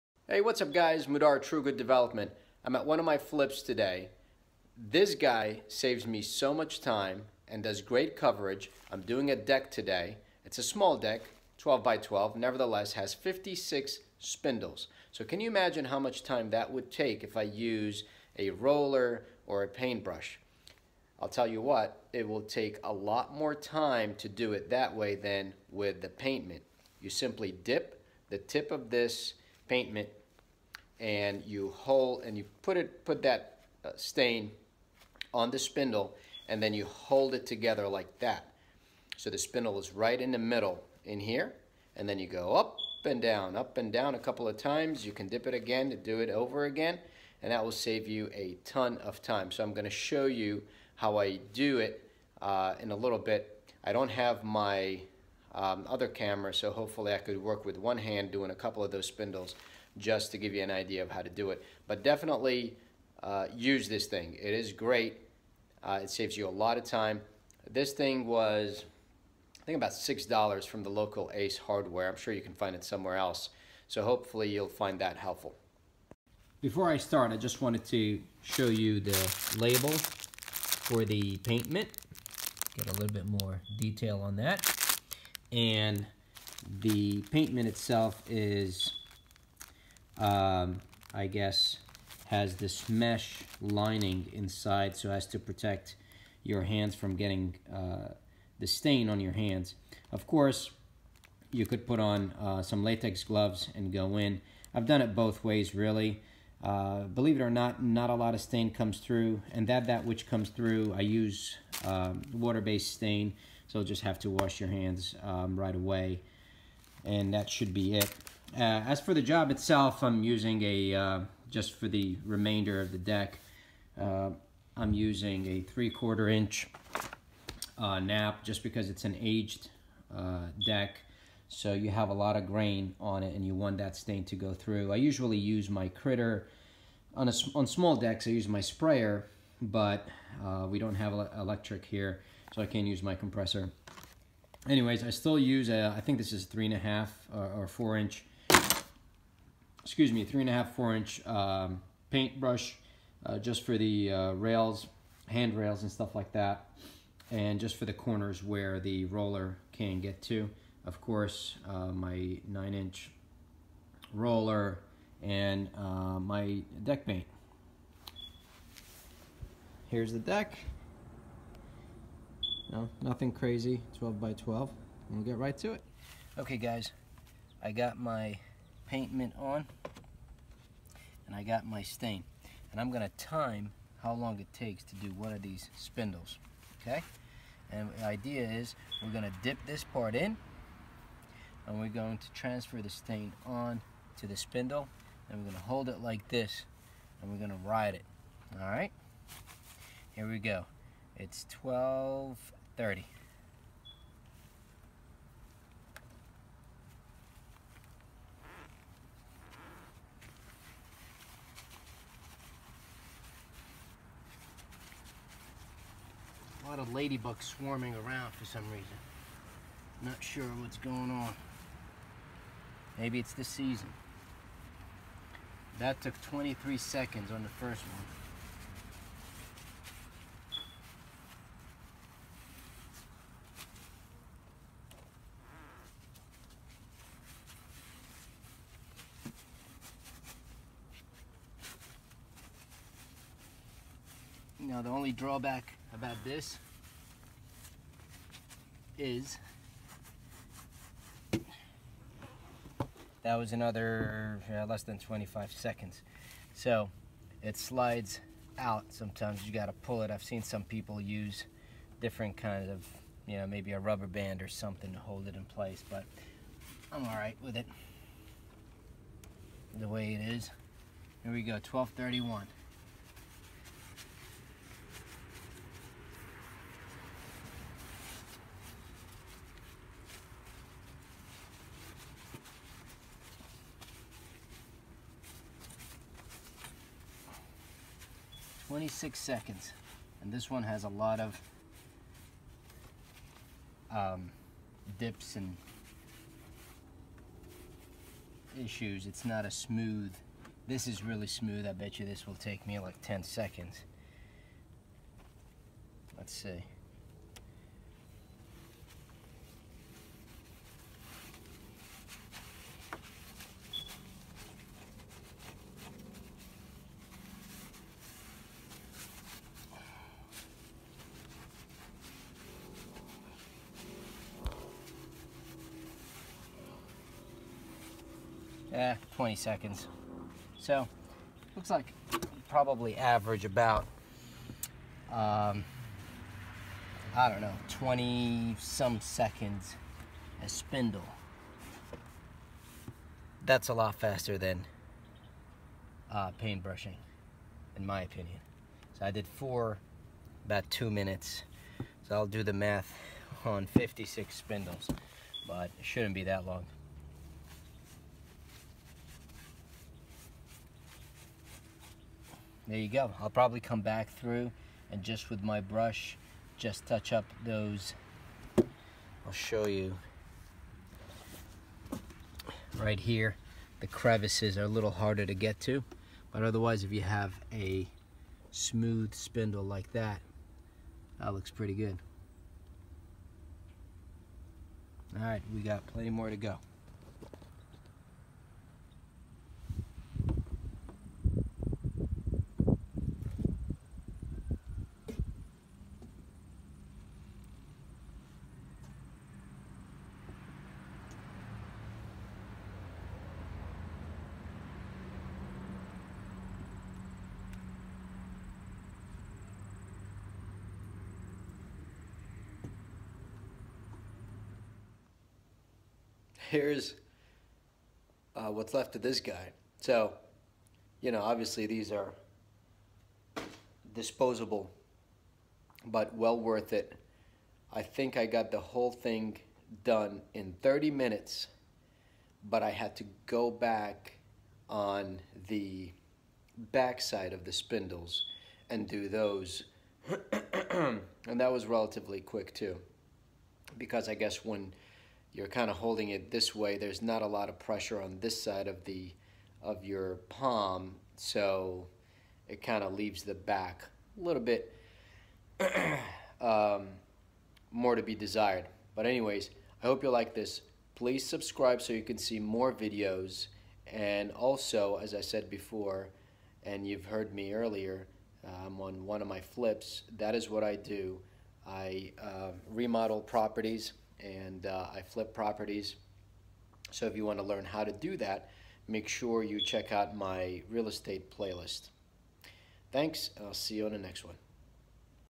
How to Stain Spindles or Paint Spindles Fast using Paint Mitt
I am at one of my recent flips and I am staining a 12 x 12 deck.  The deck has 56 spindles, so I grabbed one of my trusted tools to get the job done efficiently.  I have used the Paint Mitt on other projects and it is well worth it.  For $6 bucks, you can't beat the value.  I never tried to wash the mitts for subsequent jobs, so I can't really say whether you can or can't, try for your self and let me know.
The only draw back I have is the mitt tends to slip out of your hands, but it isn't to the point where you can't keep going.
Also, usually the back of the spindles doesn't get as much coverage as the front and sides, probably due to the nature of how the pressure is distributed when holding the spindles.  You might find it necessary to go back and hit the back of the spindles.  That shouldn't take more than a few seconds per spindle.
You may also use the mitts to paint your deck spindles.
Generally, I highly recommend using the mitt in lieu of a paint brush.